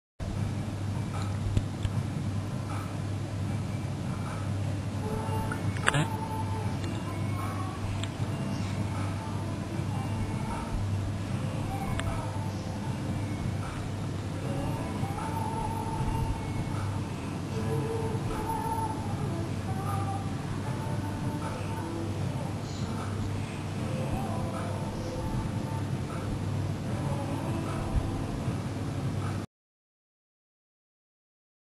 ASchatpatient
Very patient cat who is trying to eat while puppy is trying to play at the same time.